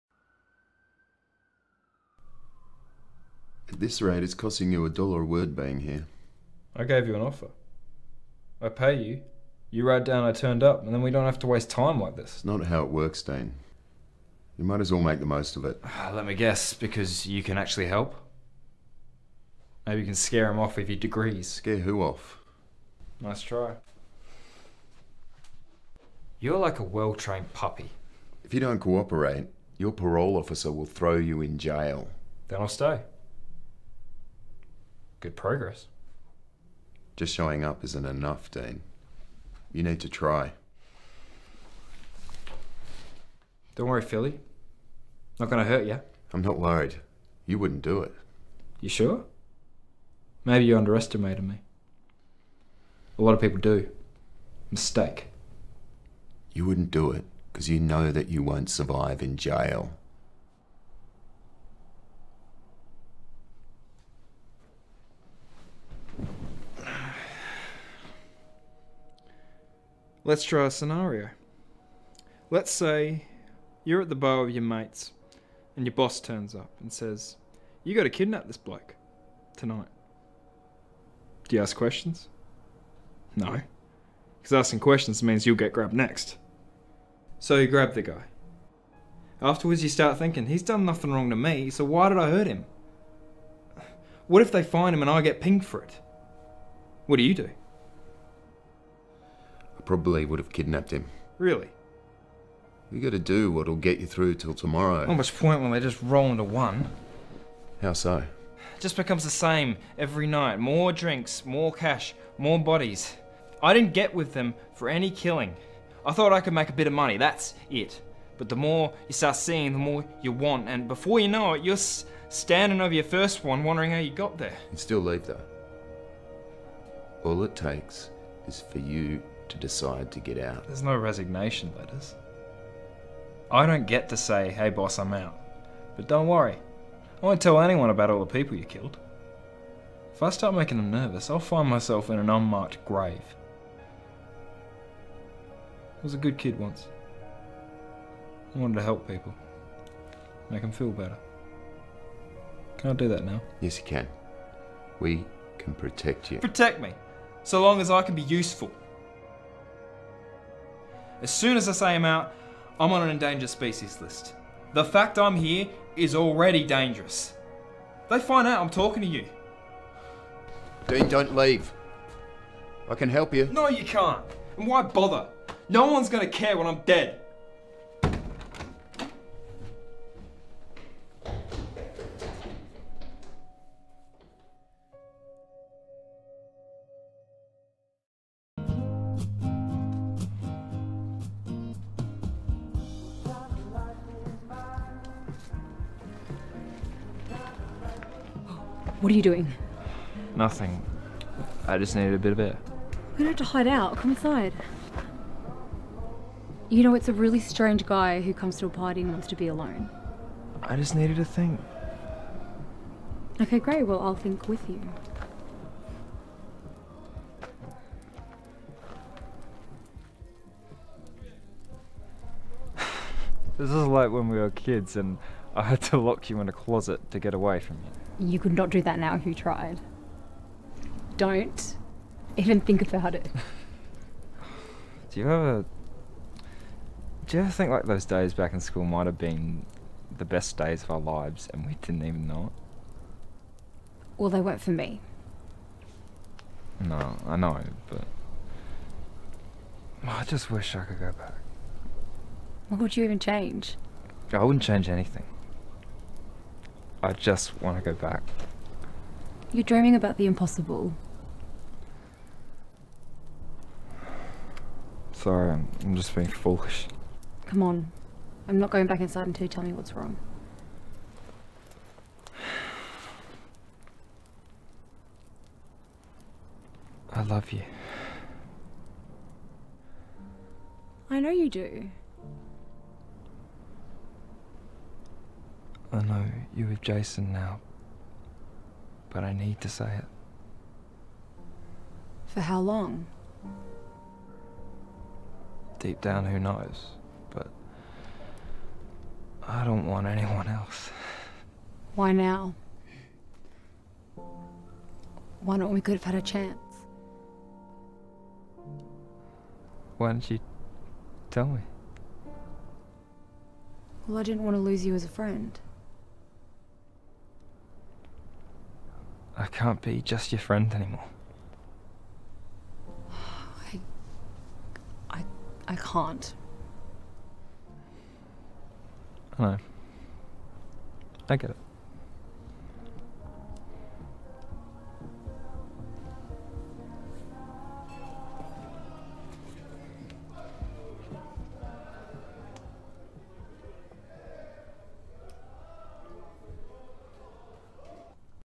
Alexander Woollatt - Slate Management
Showreel shot by Acting Showreels Melbourne
Written by Alexander Woollatt
Directed by Rohan Jones
Cast includes Camila Palacios, Michael Gwynne